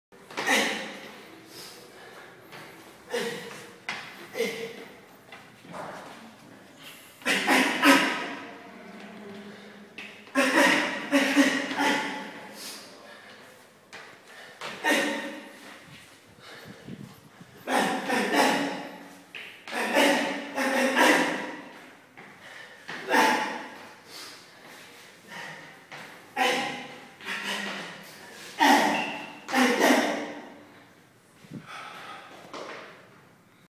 Arturos Akopian shadow boxing before the fight 01/06/2013
Event: Uppercut 51 Cyprus Fight Night
Location: AEL Basketball Stadium (Limassol, Cyprus) 1/06/2013